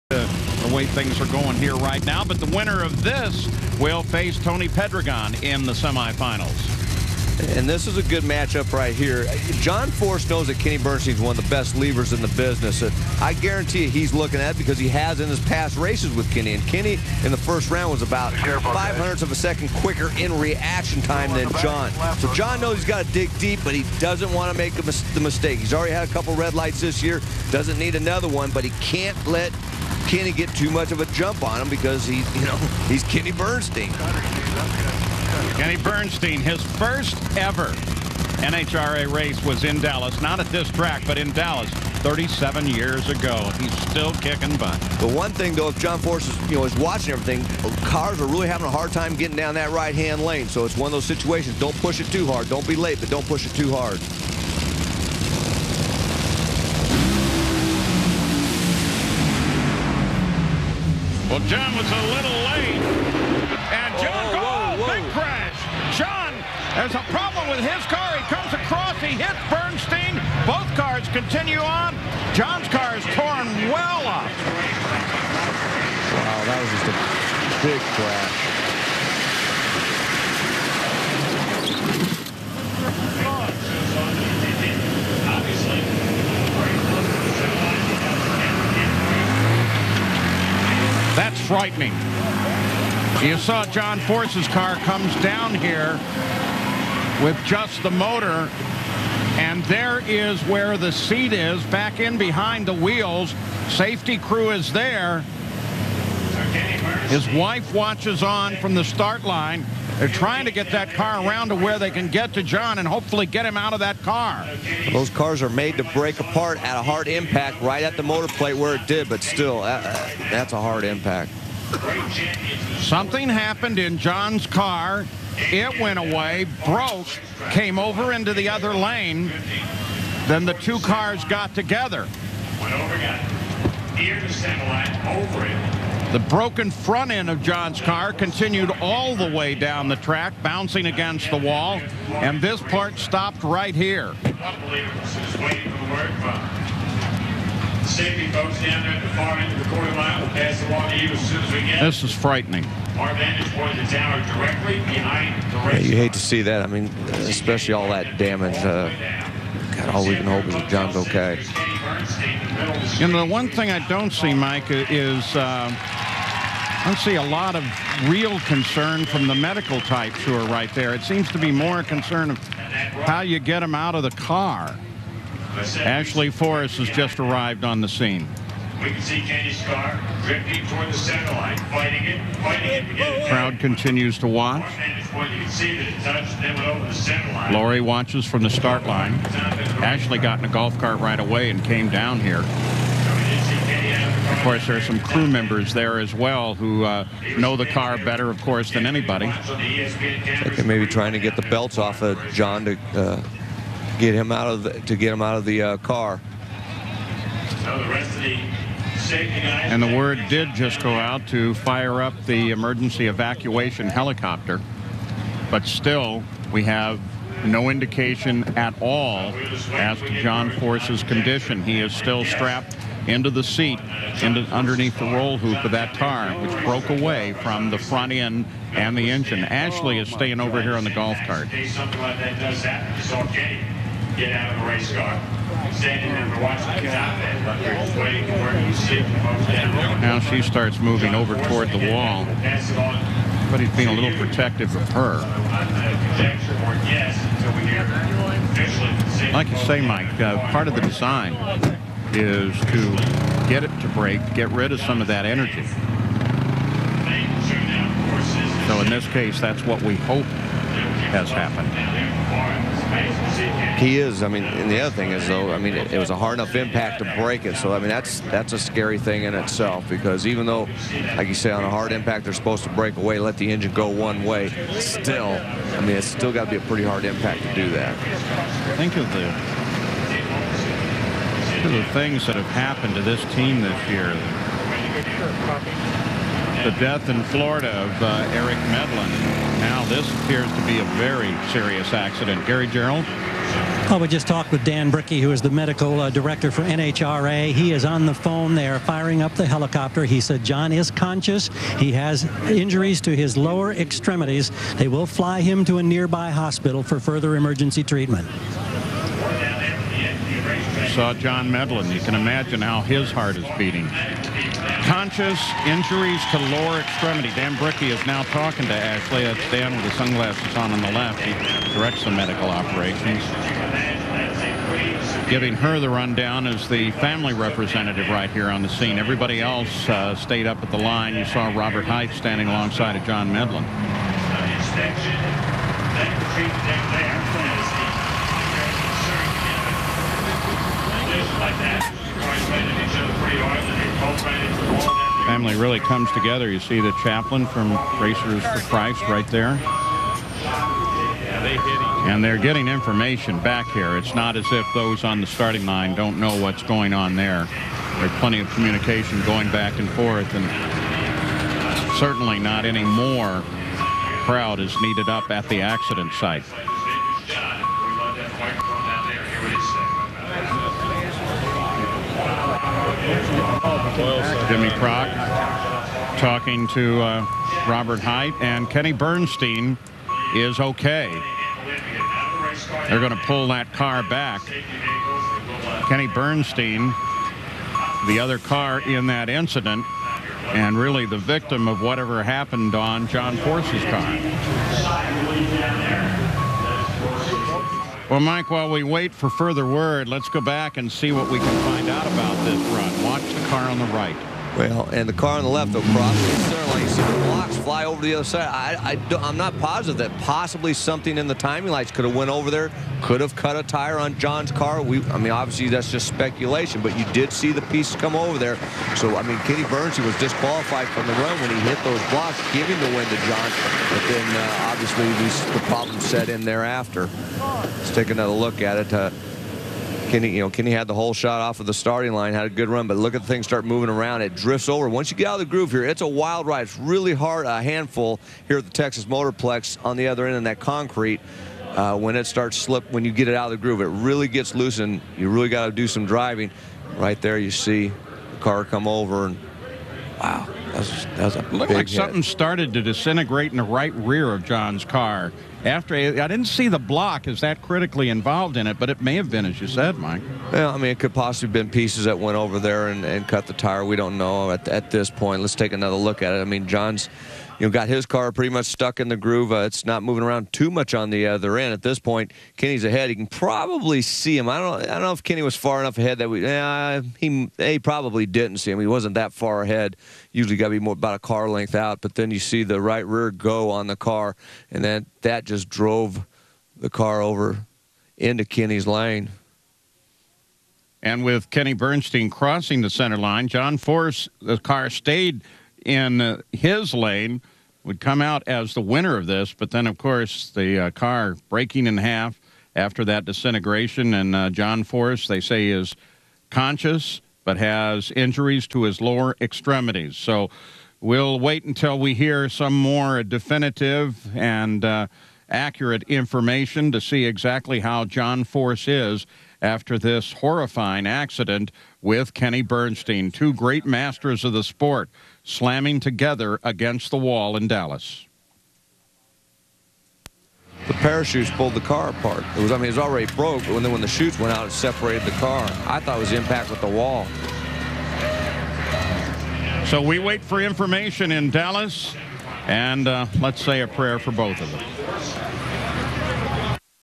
John Force Crash at Dallas -- Part 1 of 3
Coverage of the 09-23-07 Dallas race and John Force's crash.